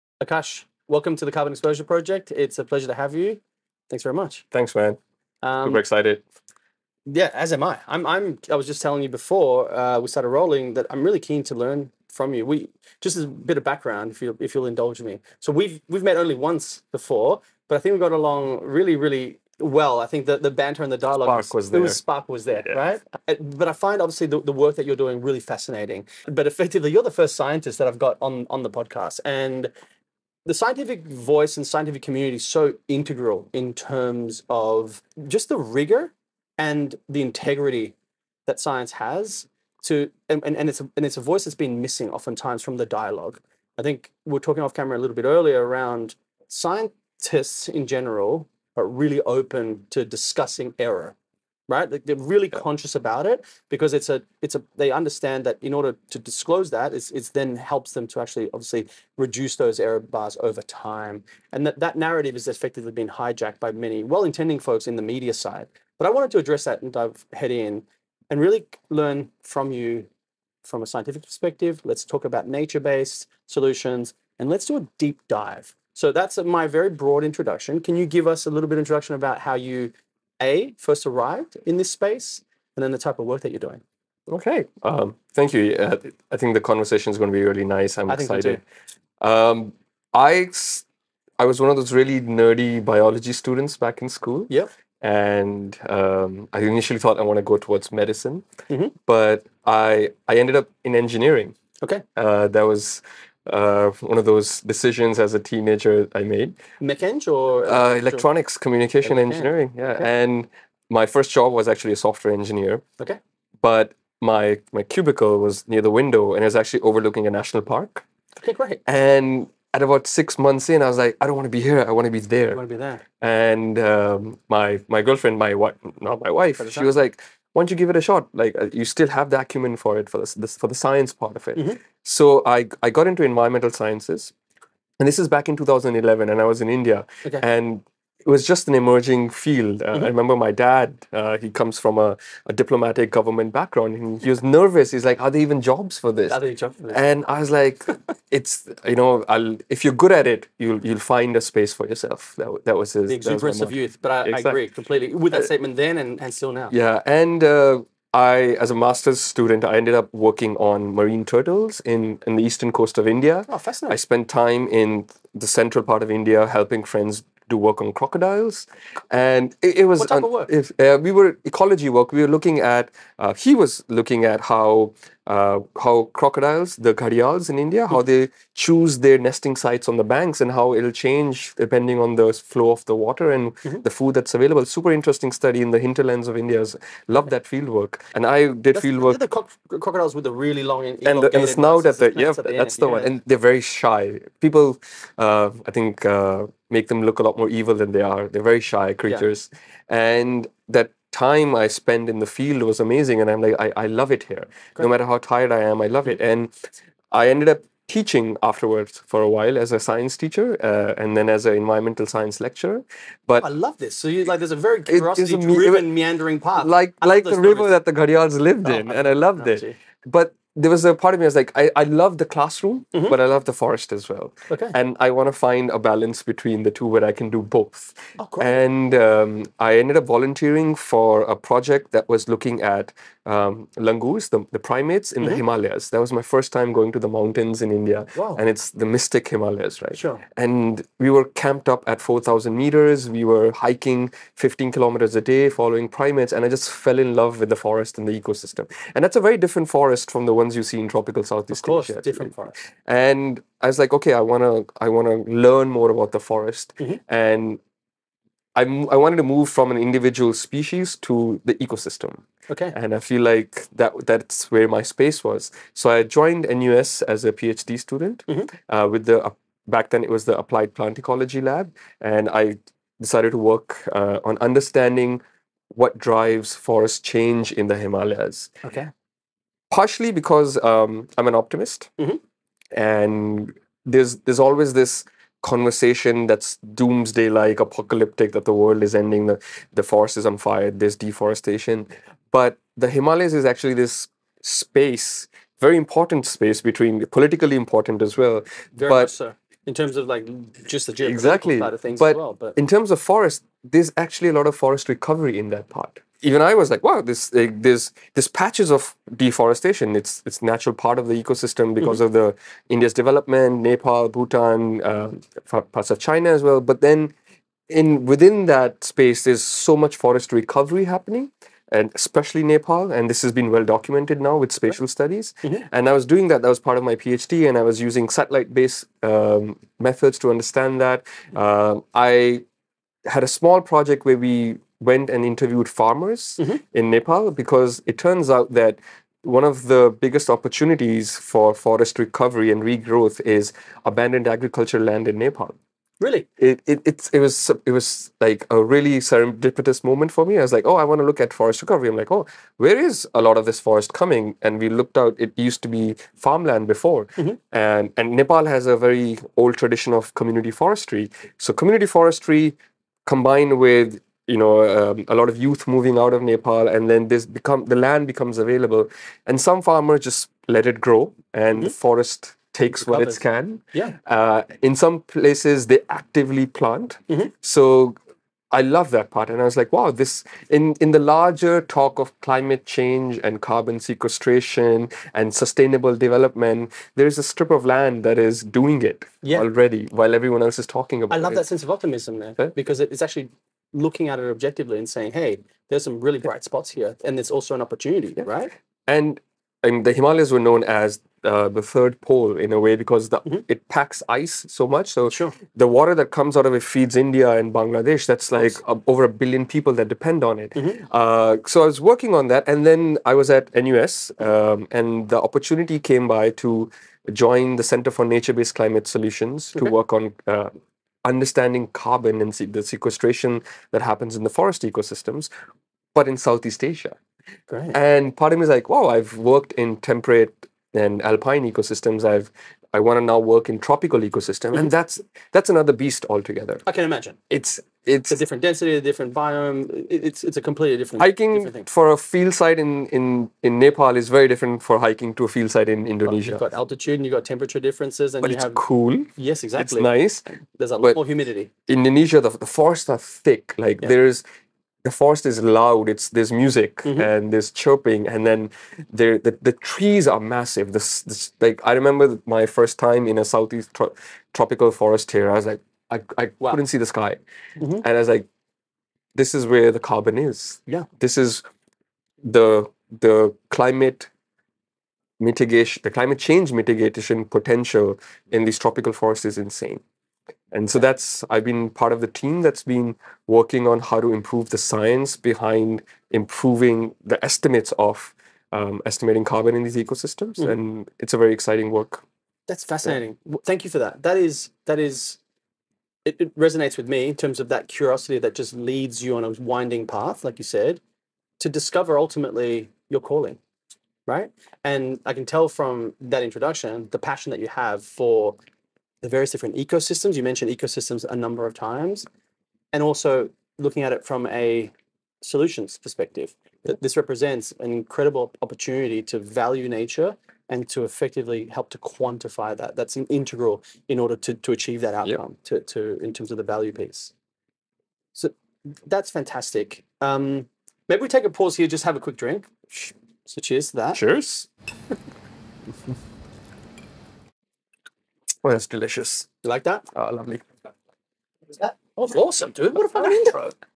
Exploring Himalayan and Southeast Asian Ecosystems with Akash Verma | Episode 8 (Part 1)
Join us for the first episode of our (3-part) interview with Akash Verma, a Postdoctoral Research Fellow at the National University of Singapore.

In this first part episode, Akash shares his unique insights into the fascinating world of forest recovery in the Himalayas and the vibrant tropical ecosystems of Southeast Asia.

He also talks about his journey as a scientist and how it has added depth to his understanding of nature's role in climate solutions.

Timestamp:
00:35 broad introduction about why a scientist adds value to the podcast
01:40 Introducing his path throughout his career
05:20 Talking about forest recovery in the Himalayas
07:30 Tropical Ecosystems in Southeast Asia